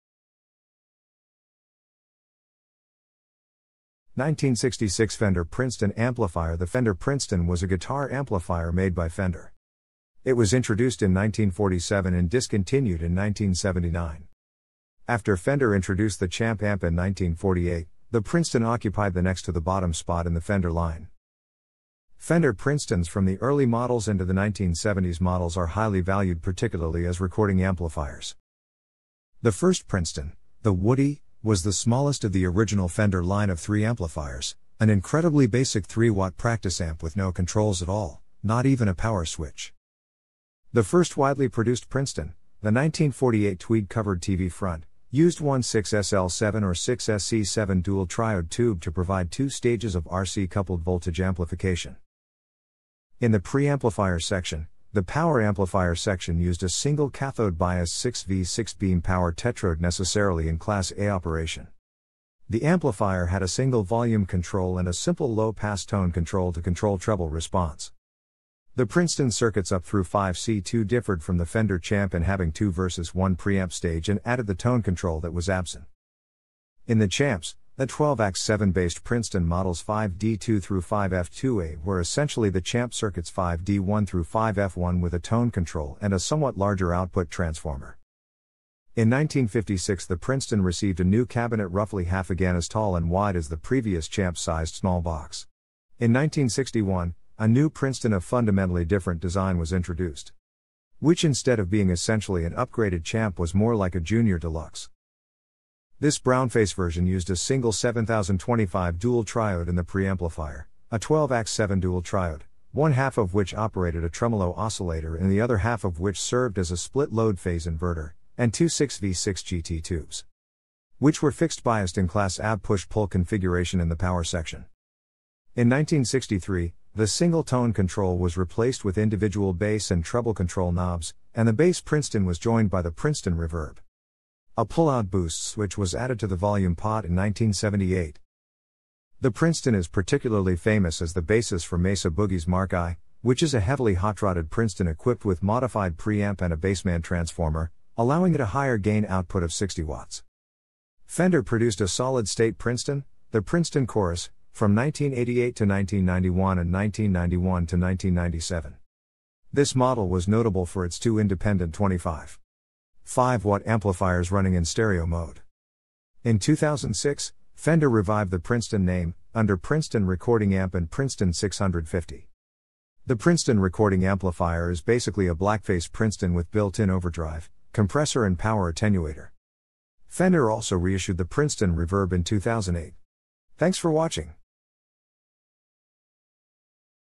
Fender Princeton
1966 Fender Princeton amplifier The Fender Princeton was a guitar amplifier made by Fender.
It was introduced in 1947 and discontinued in 1979.
After Fender introduced the Champ Amp in 1948, the Princeton occupied the next to the bottom spot in the Fender line.
Fender Princetons （as well as their sister amp the Princeton Reverb） from the early models into the 1970s models are highly valued particularly as recording amplifiers.
The first Princeton, the "Woody" （so called for its uncovered wooden cabinet）, was the smallest of the original Fender line of three amplifiers, an incredibly basic 3-watt practice amp with no controls at all, not even a power switch.
The first widely produced Princeton, the 1948 tweed-covered "TV front," used one 6SL7 or 6SC7 dual-triode tube to provide two stages of RC-coupled voltage amplification
in the preamplifier section; the power amplifier section used a single cathode-biased 6V6 beam power tetrode necessarily in Class A operation.
The amplifier had a single volume control and a simple low-pass tone control to control treble response.
The Princeton circuits up through 5C2 differed from the Fender Champ in having two versus one preamp stage （6SC7 dual-triode vs 6SJ7 pentode） and added the tone control that was absent
in the Champs; the 12AX7-based Princeton models 5D2 through 5F2-A were essentially the Champ circuits 5D1 through 5F1 with a tone control and a somewhat larger output transformer.
In 1956 the Princeton received a new cabinet roughly half again as tall and wide as the previous Champ-sized "small box." In 1961, a new Princeton （6G2） of fundamentally different design was introduced,
which instead of being essentially an upgraded Champ was more like a junior Deluxe.
This "brownface" version used a single 7025 dual triode in the preamplifier; a 12AX7 dual triode, one half of which operated a trem...